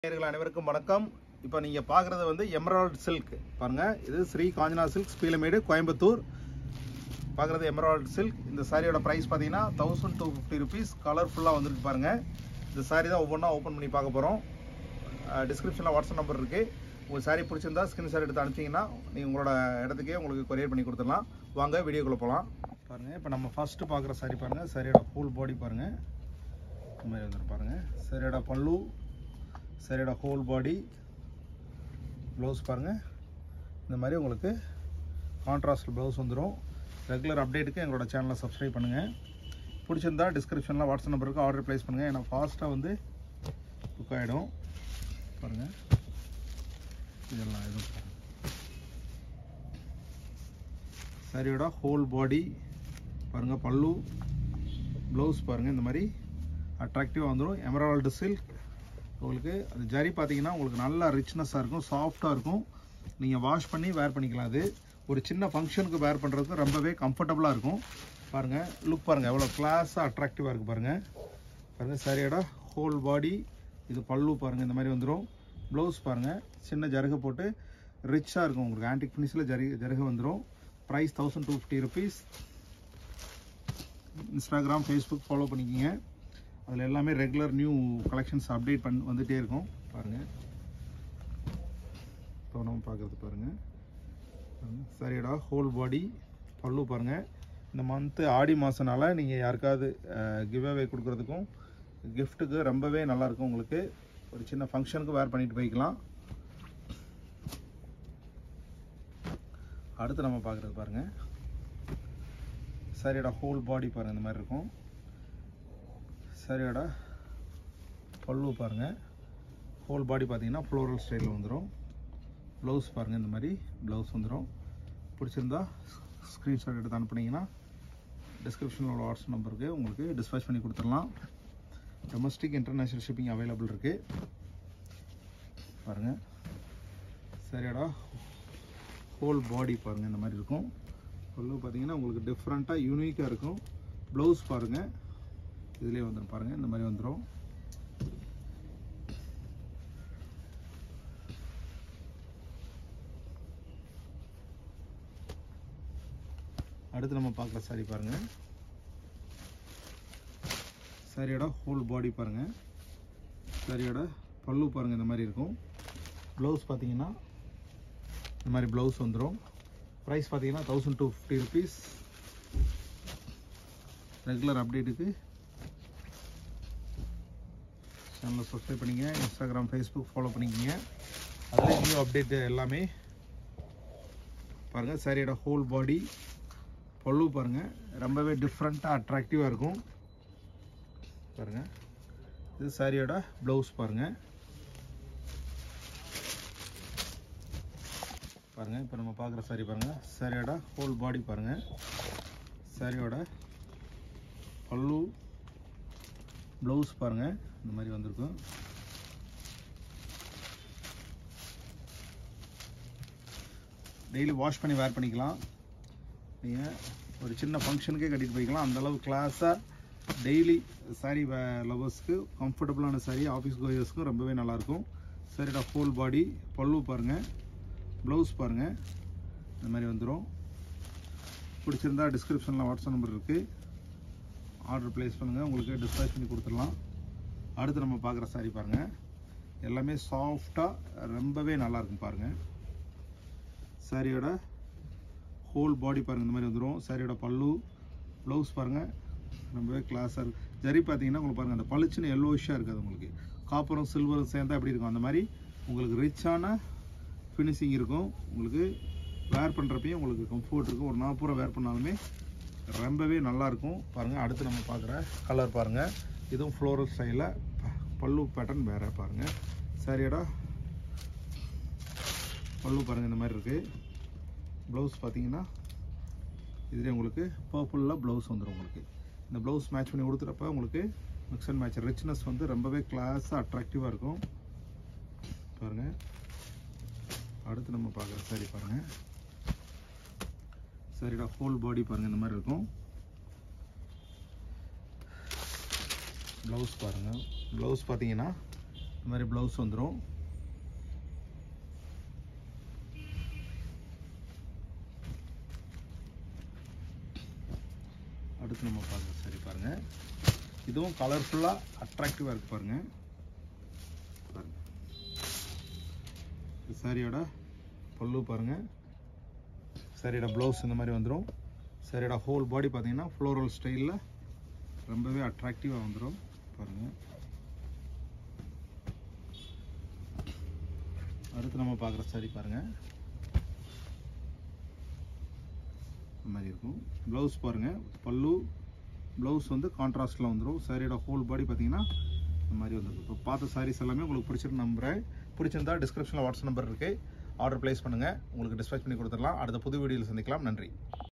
EMARLDsilk saree (new arrival) only@1250🏷️ @ loom price- NEWLY ARRIVED
SAREE DETAILS:
Emarld silk saree -newly arrived only @1250🏷️
With various colours
This video is based on specific saree advertisement
SHOP DETAILS:
Sri kanchanaa silks,
No-14.A Lal Bahadur colony,
Near gopalnaidu hr.sec.school,
Peelamedu,
Coimbatore-4